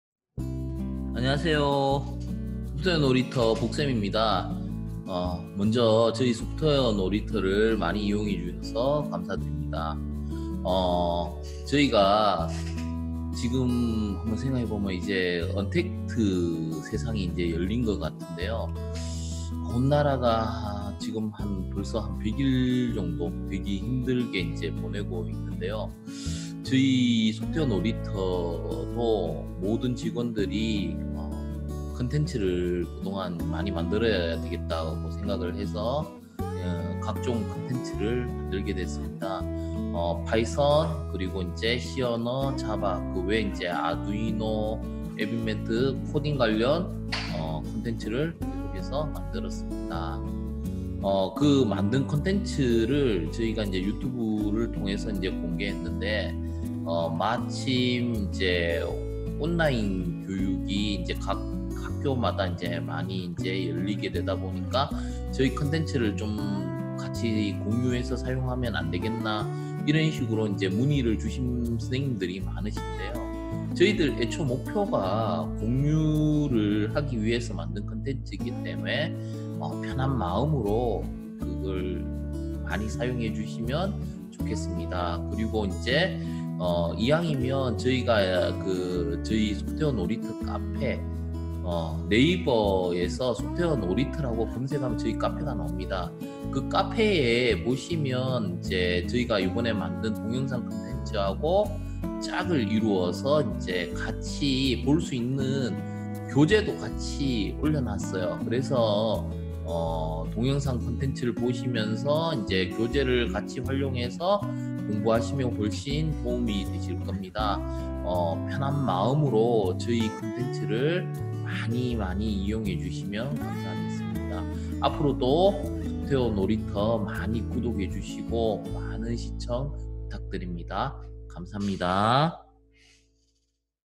소놀 교육 자료 공유에 대하여~ (여러분~! 감사합니다.)
오늘은 그동안 많이 질문해주셨던 소놀 교육 자료 공유에 대한 이야기를 짧게 찍어보았습니다.

소놀 유튜브에 있는 동영상 강의는 물론
소놀 카페에 있는 다양한 교육 자료들까지
교육을 위한 목적이라면 누구든 활용하실 수 있답니다. ^^
(단, 상업적 수단을 위한 활용은 안된답니다. 사용하실 때에는 출처도 꼭 함께 남겨주세요~^^)

바이러스 사태로 인해 교육업이 많이 힘들어지고 있지만
다 함께 이겨내보아요.
항상 소놀을 찾아주시는 모든분들께 다시 한번 진심으로 감사드립니다.

(인코딩 문제로 영상 약간 화질이 줄었답니다. ㅜㅜ
양해 부탁드려요~)